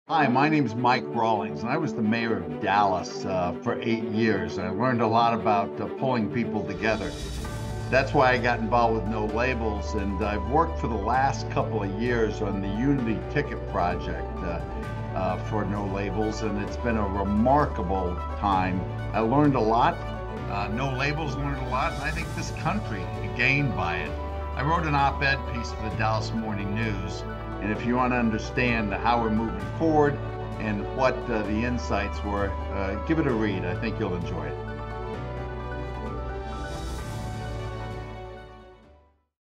Mike Rawlings On His Recent Op-Ed in The Dallas Morning News
Former Dallas Mayor Mike Rawlings on the Unity ticket project: “I learned a lot. No Labels learned a lot. And I think this country gained by it.”

Find No Labels on Facebook: Facebook.com/NoLabels
Check us out on Instagram